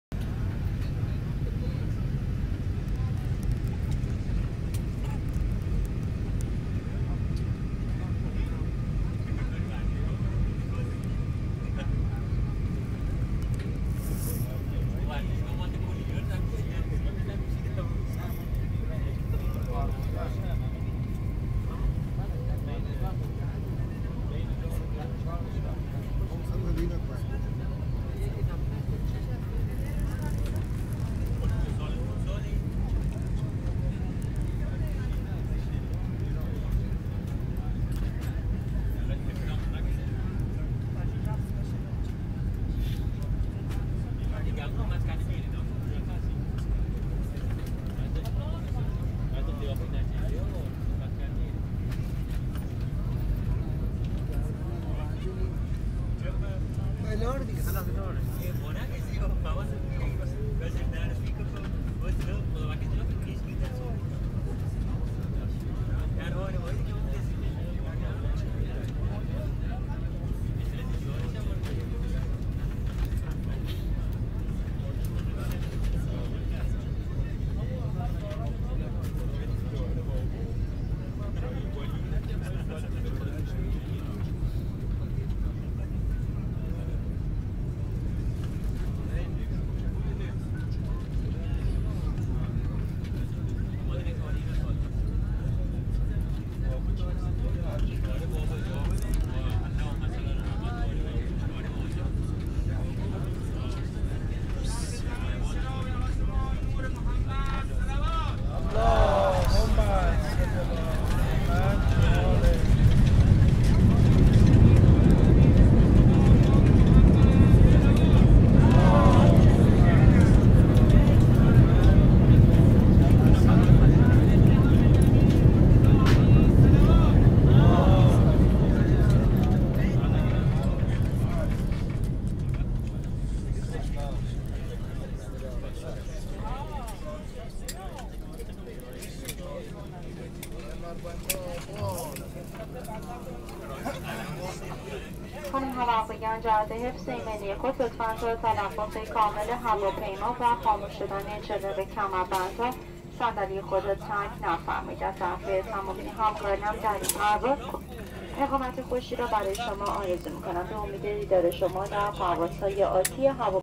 لندینگ هواپیمای airbus 320 iranair در فرودگاه بین المللی لارستان
لندینگ هواپیمای ایرباس a320 هواپیمایی iranair از مقصد کویت در فرودگاه بین المللی لارستان فارس
31/03/2017
11/01/1396